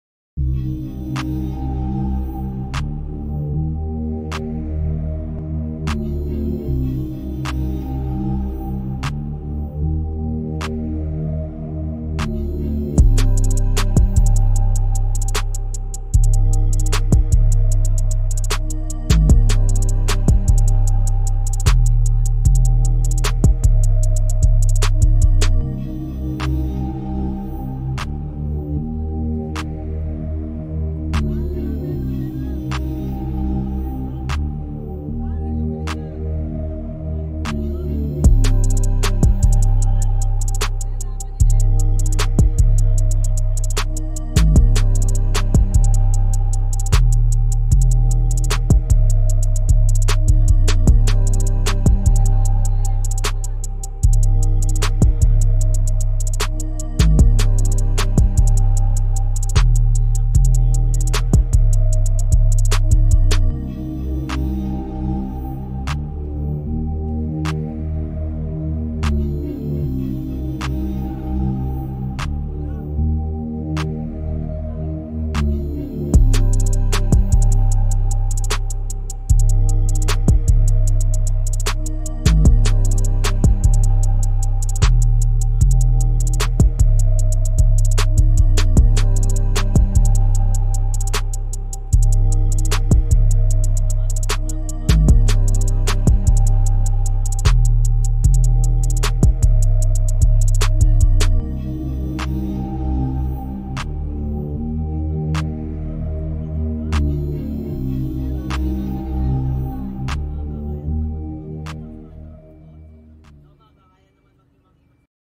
Patok na Street Foods, masarap ito! Mabenta sa masang Pilipino.
Street foods masarap na, mabenta sa masang Pinoy. Malinis po ito, ito ay patok na patok sa mga trabahador. Kain muna bago byahe para busog.